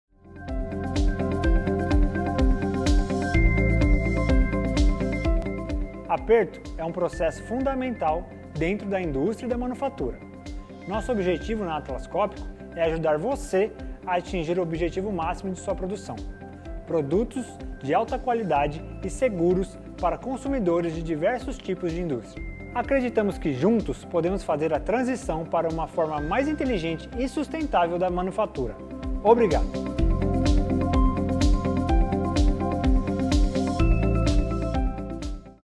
Parafusadeira à bateria Tensor IxB e outras soluções da Atlas Copco para a Indústria 4.0
Aperto é um processo fundamental nas linhas de montagem e por isso que a Atlas Copco investe cada vez mais em soluções inovadoras para acompanhar a revolução da Indústria 4.0. Juntos, podemos fazer a transição para uma forma mais inteligente e sustentável da manufatura. Seja com a Tensor IxB, seja com outras soluções do nosso portifólio, estamos à disposição para ajudar sua equipe a atingir aos objetivos de produção e elevar os padrões de qualidade cada vez mais.
Acompanhe outros lançamentos, cases, soluções e guias da Atlas Copco no Blog Fábricas Inteligentes

Fale com um especialista Atlas Copco e conheça as soluções do nosso portifólio.